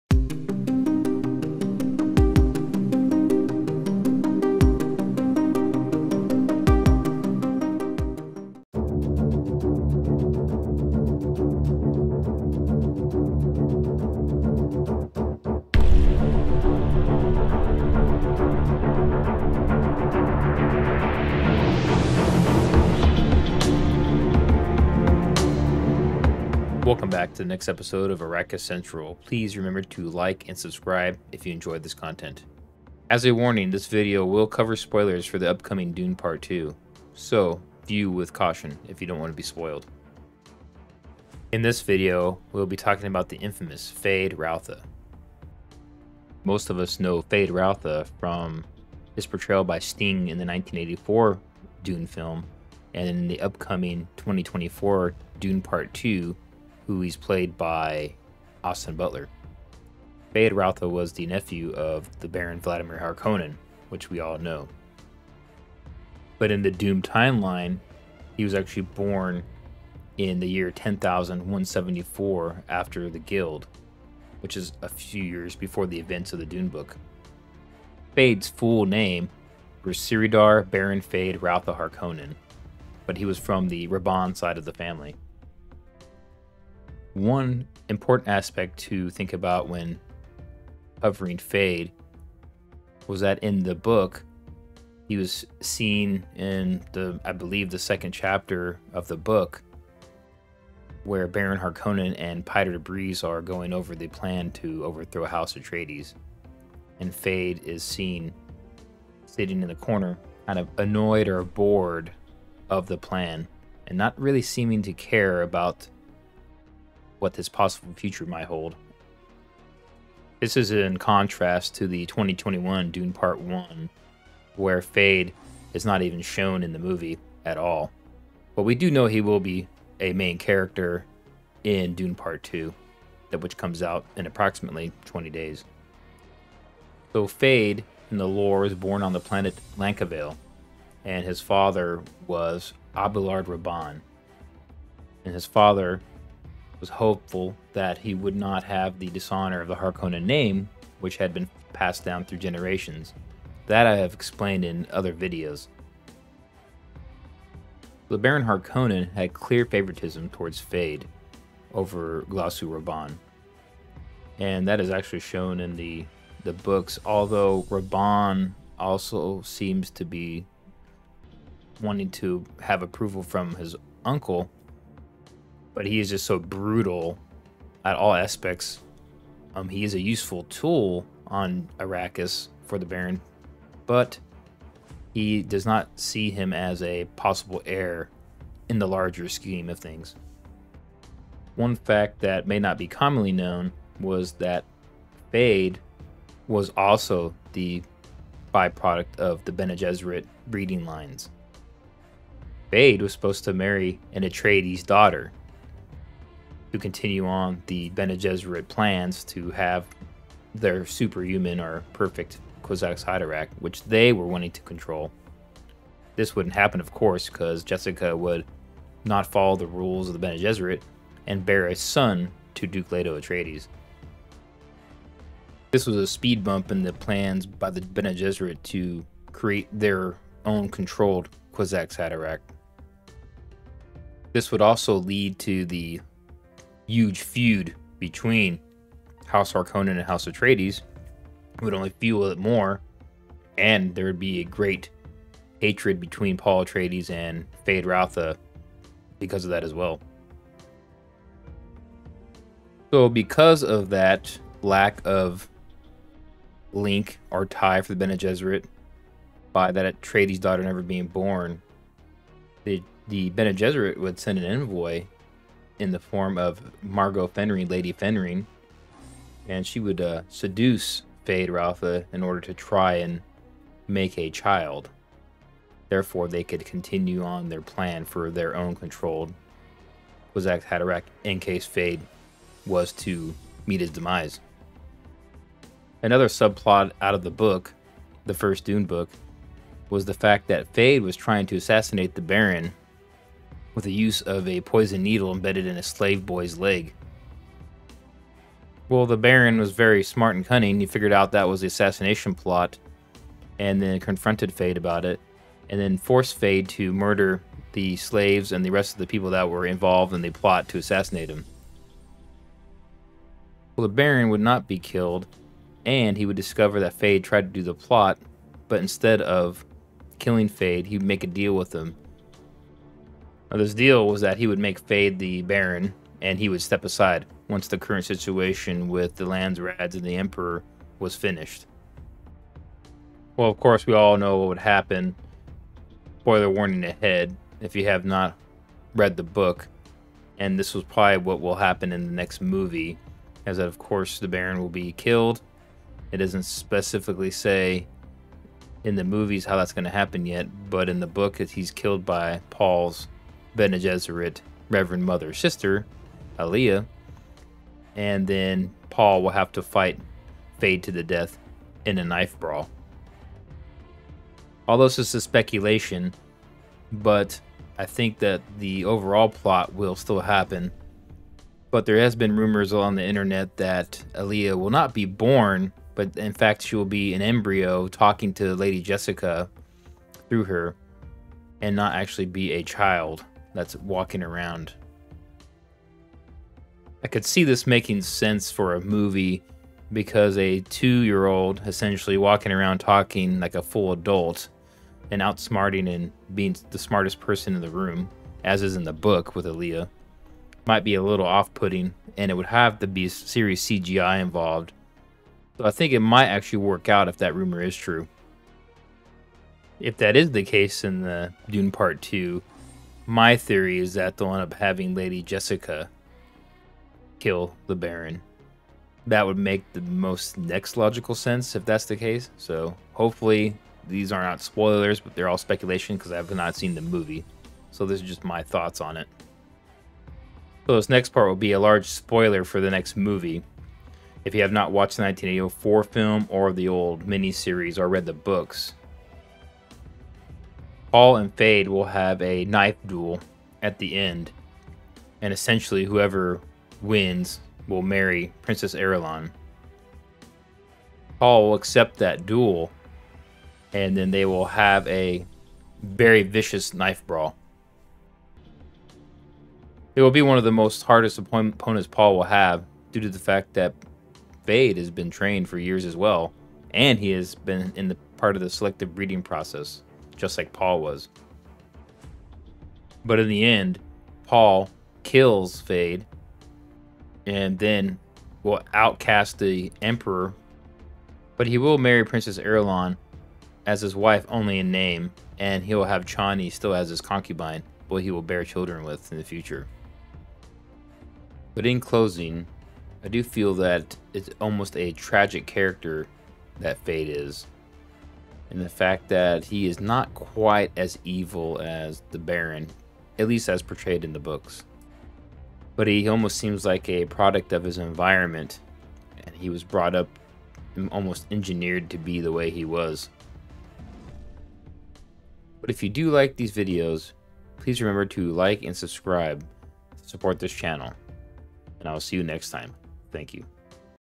Dune: What Are Feyd's Origins?
Welcome to my new channel, Arrakis Central. In this video we will talk about Feyd Rautha, and what happenes to him during the events of Dune.